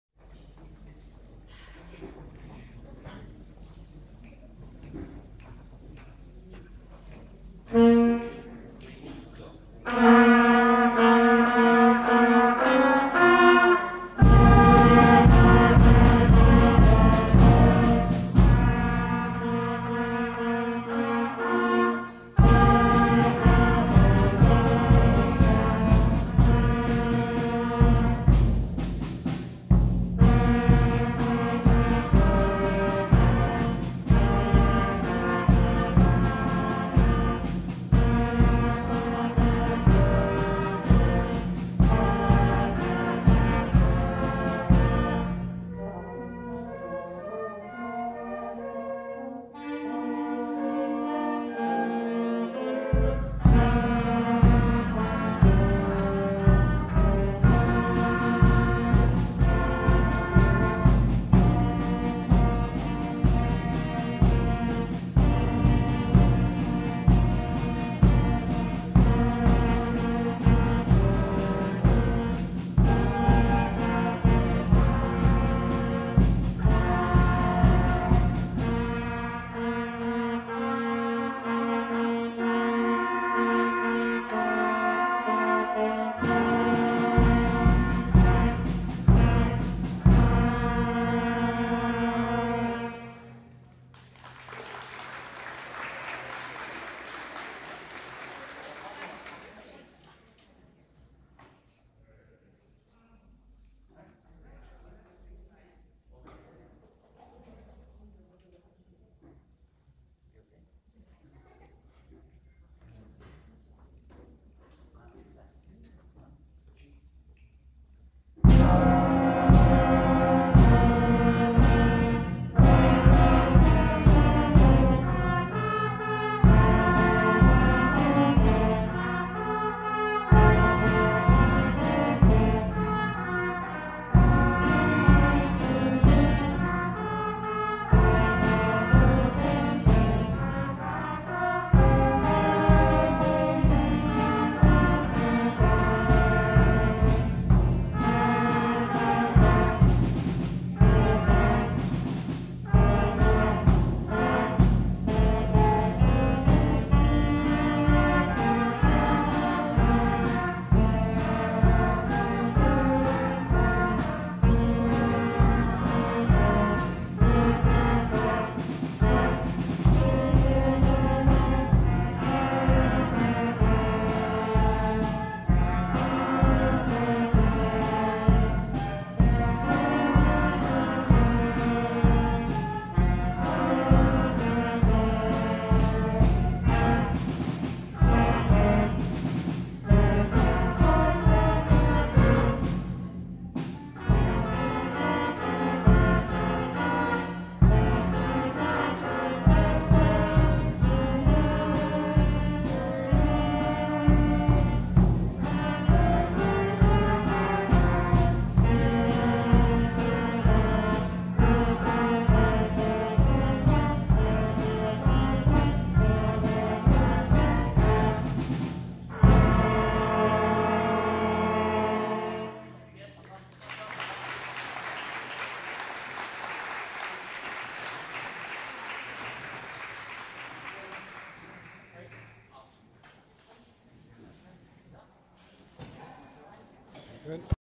Jeanne Sauve FIPS Grade 8 band - TVCHSA AGM 2015
Thames Valley Council of Home & Schools AGM 2015 - The Jeanne Sauve French Immersion Public School grade 8 band providing some wonderful early evening entertainment